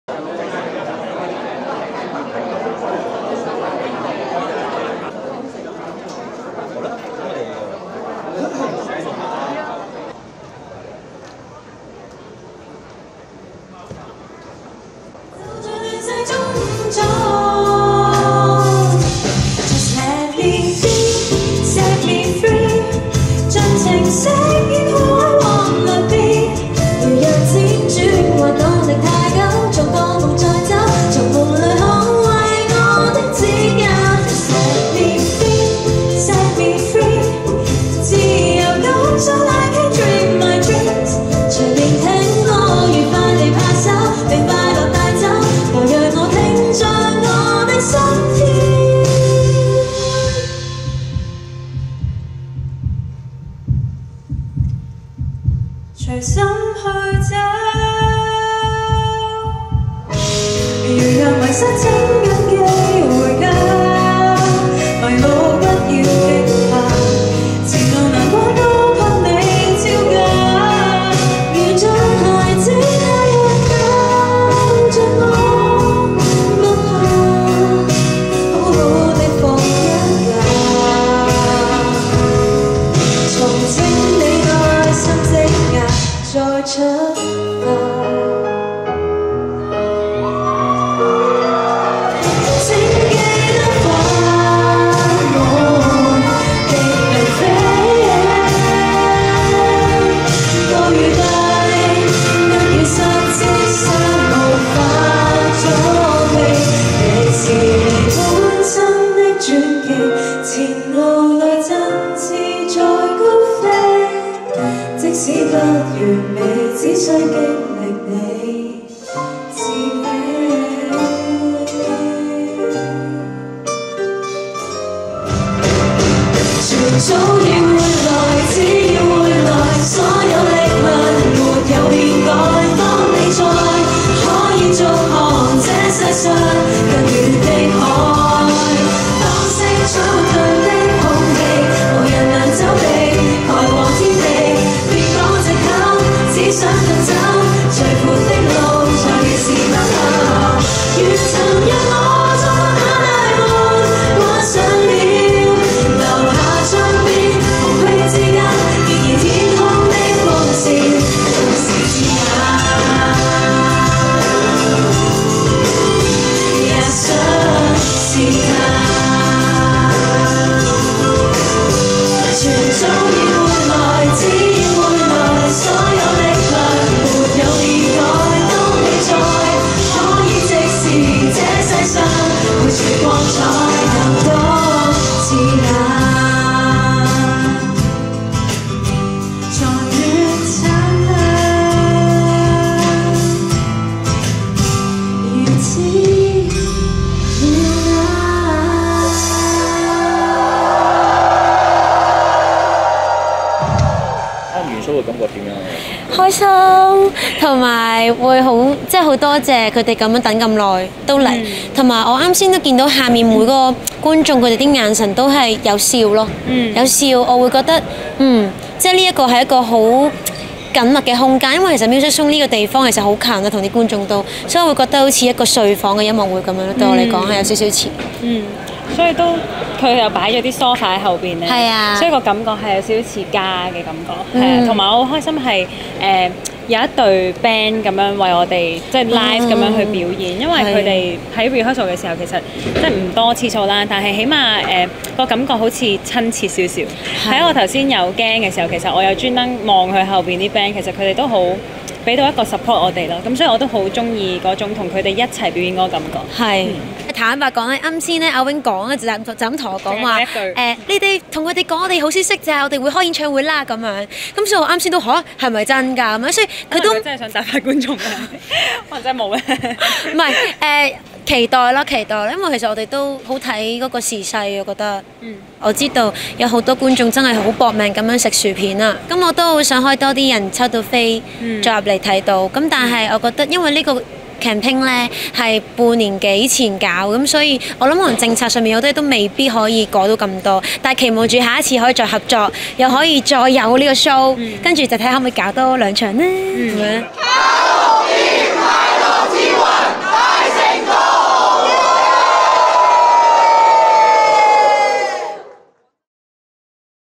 【ACG狂熱】日知名零食×港人氣女團! 追尋"塊"樂之魂!
【ACG狂熱】日知名零食@CalbeeJp×港人氣女團@COLLARweare! 追尋"塊"樂之魂!

Calbee 卡樂B # Calbee追尋你的塊樂之魂音樂會 @calbee3833